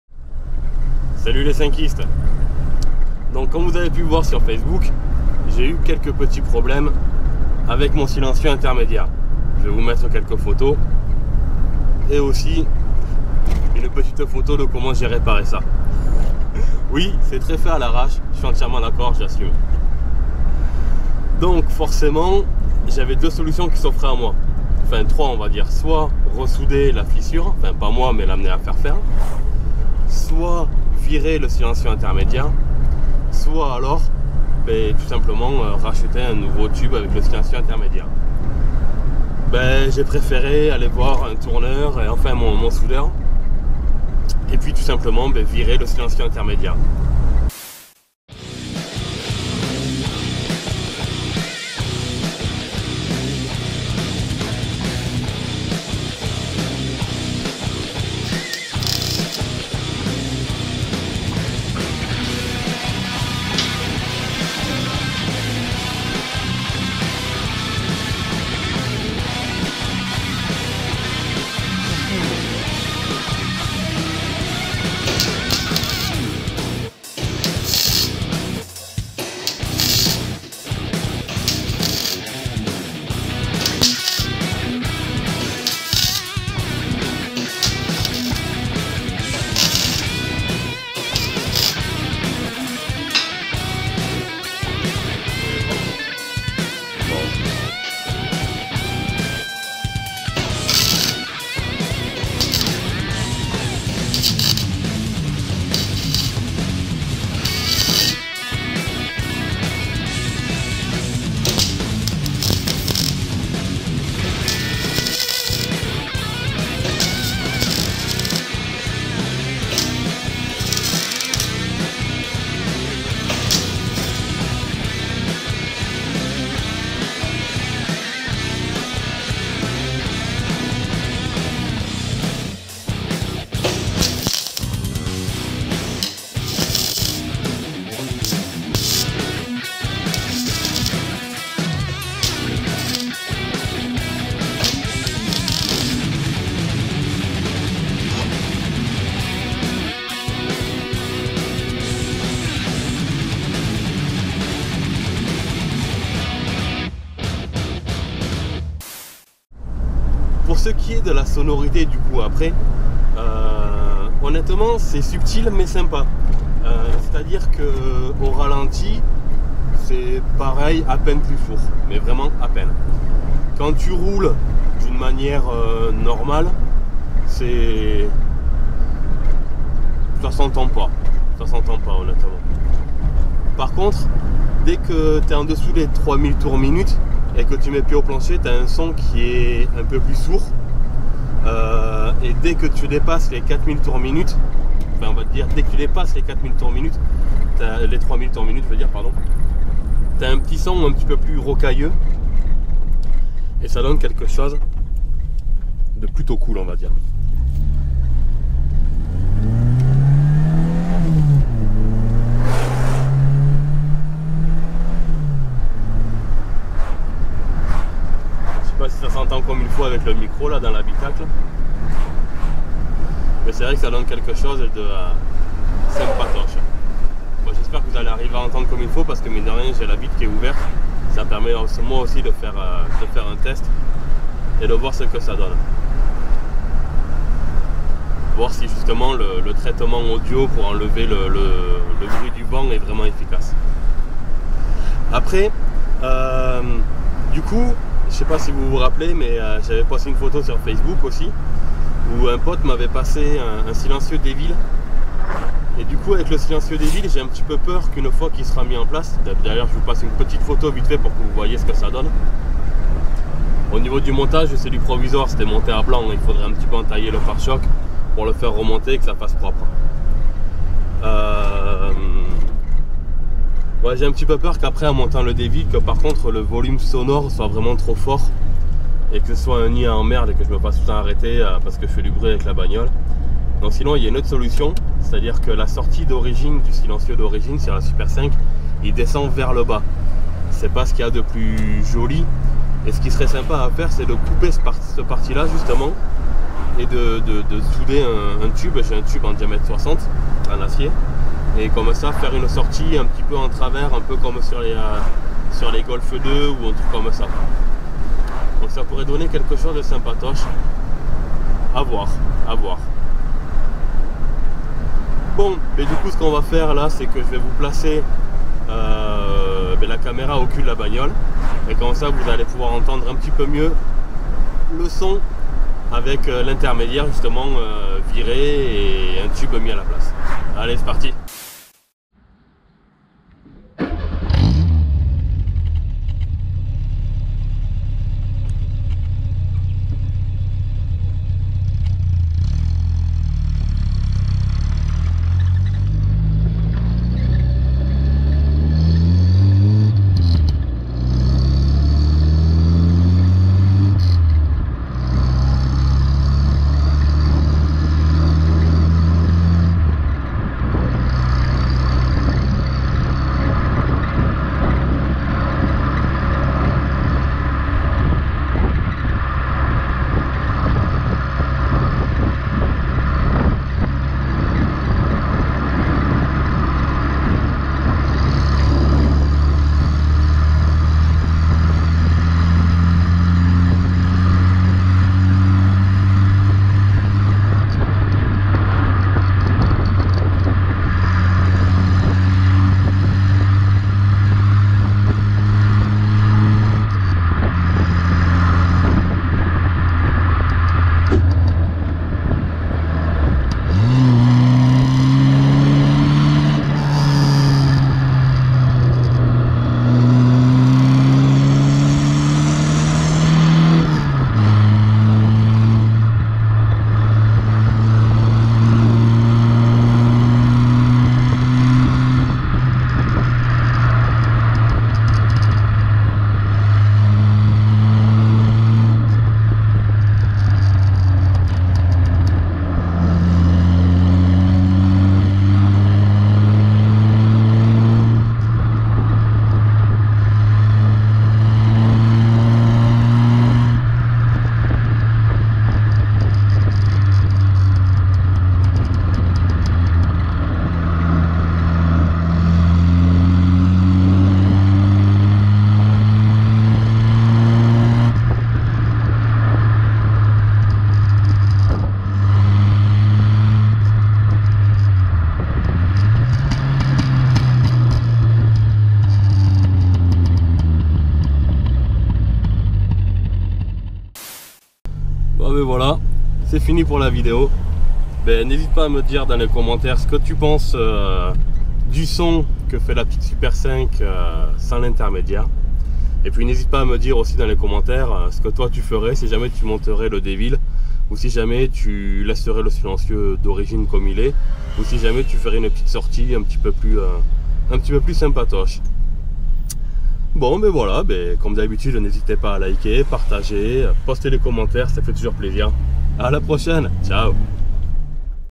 Echappement super 5 GTS partie 1
Après un petit problème avec le silencieux intermédiaire, plus de problème car plus de silencieux ^^